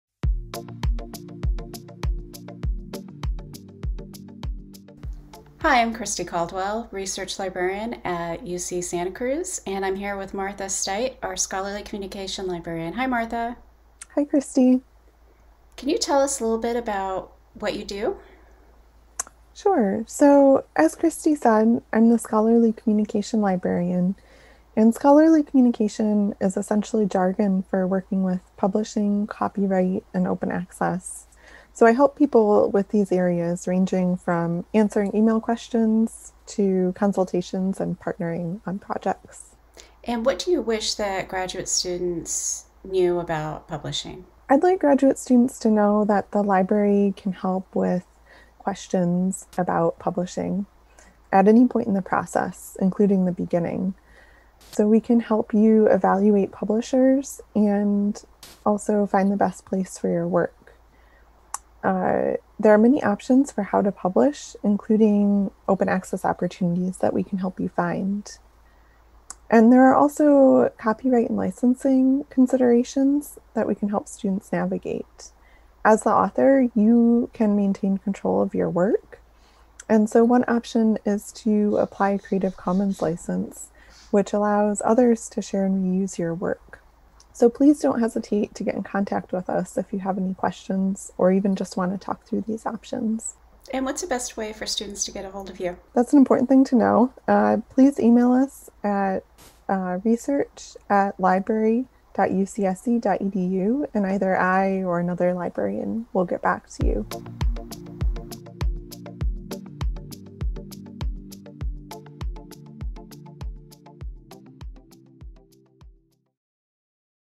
In Conversation, Martha Stuit, Scholarly Communication
Martha Stuit, UCSC Scholarly Communication Librarian, explains what new graduate students should know about publishing and their author rights.